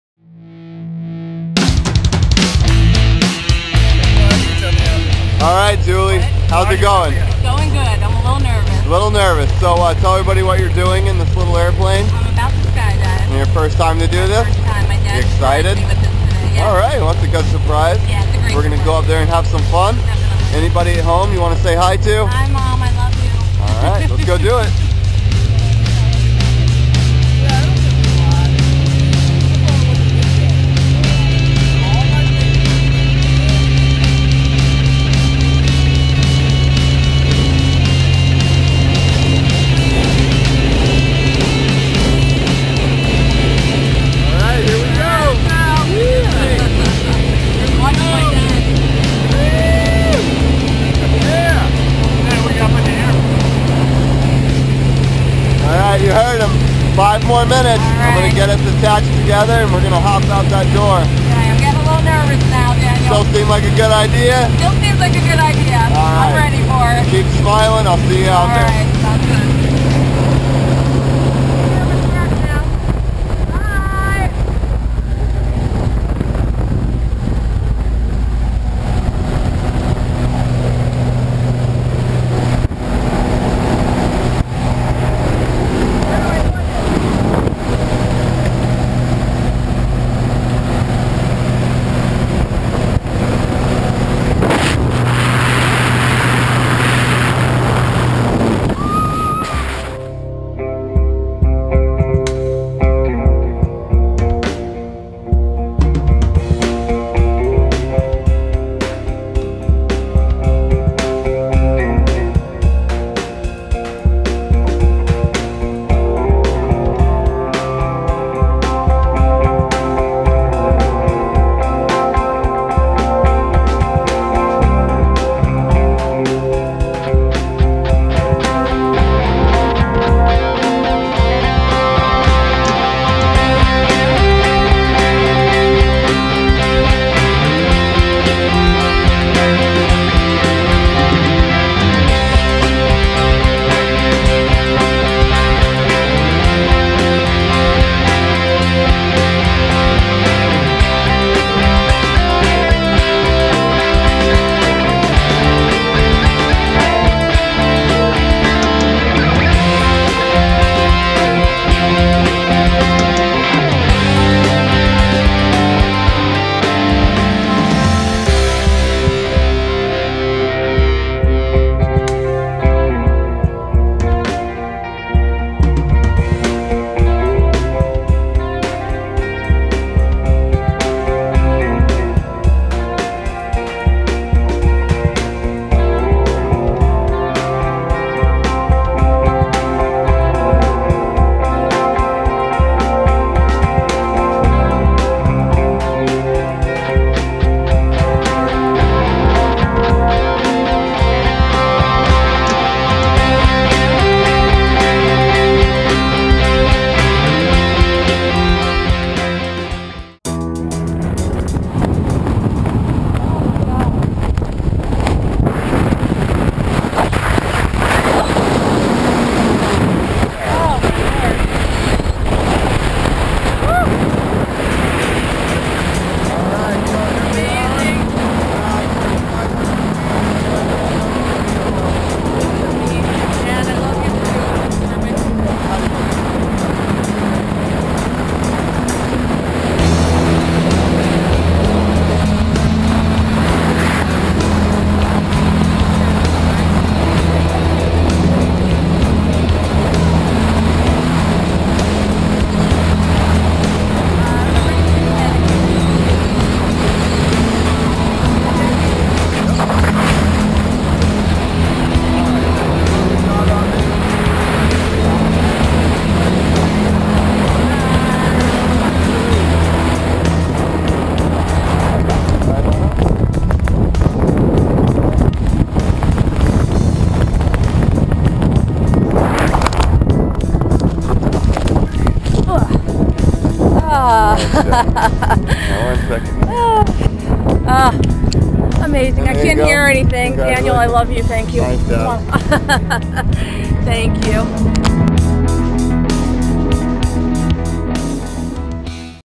Julie Bales
Tandem Skydiving in Maryland over Ocean City - Julie Bales